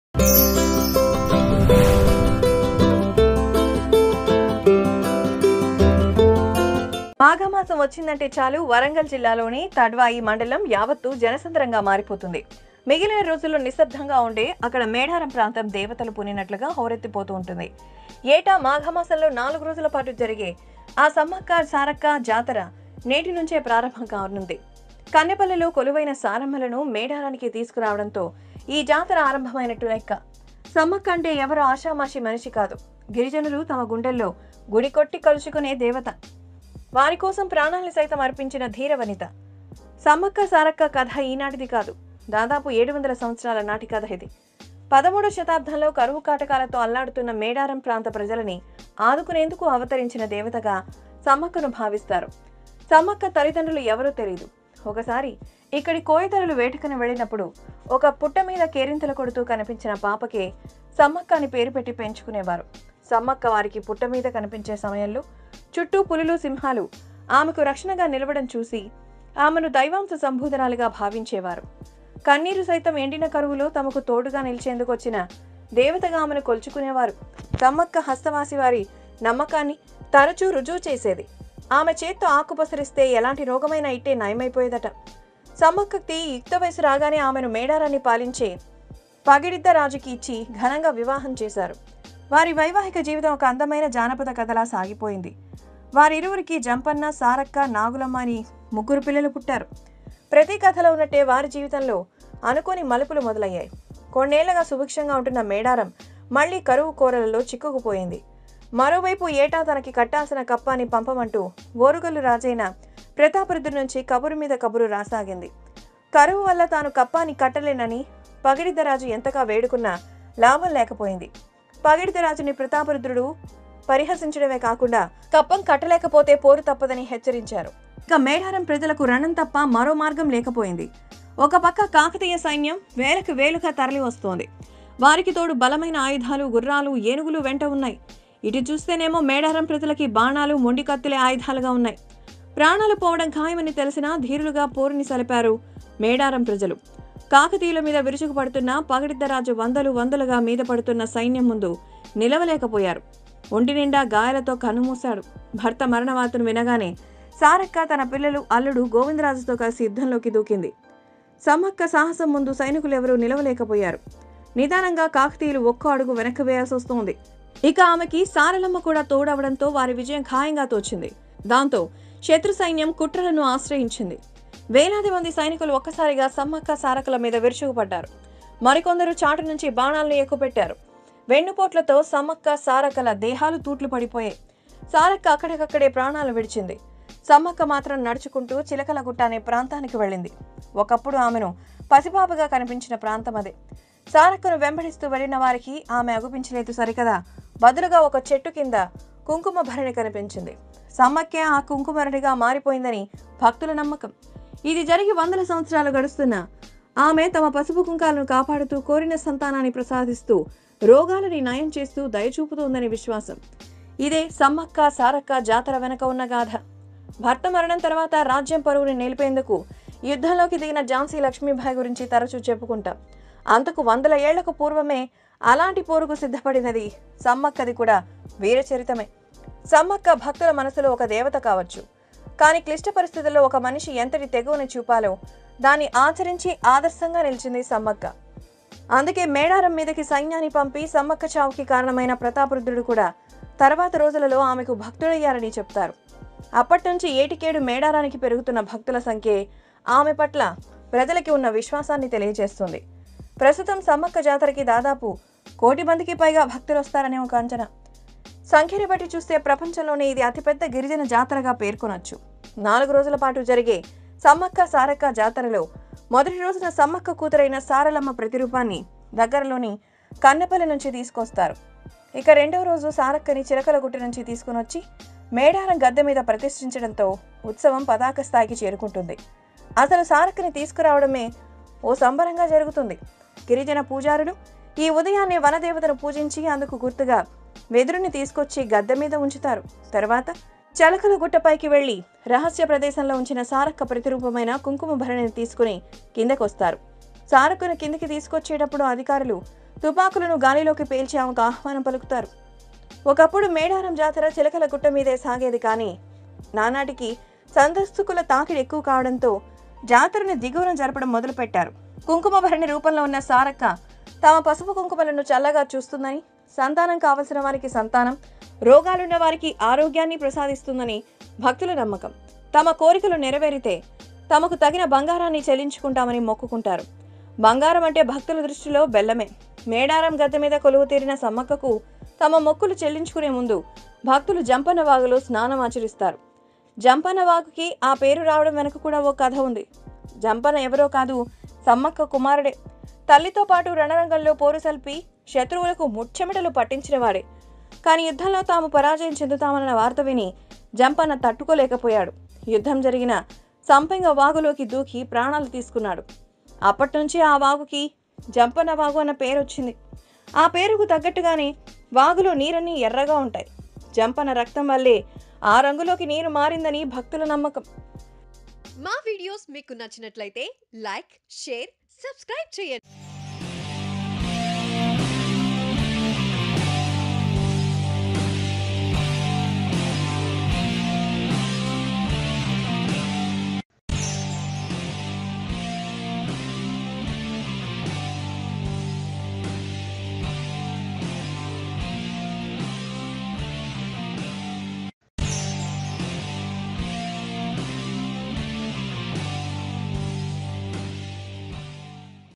Sammakka Sarakka Full Story | Real Story Of Sammakka Saralamma | Devotional Stories | News Mantra
Sammakka Sarakka Full Story. The real story of Hindu Goddesses Sammakka and Sarakka (Sammakka and Saralamma) can be known in this video.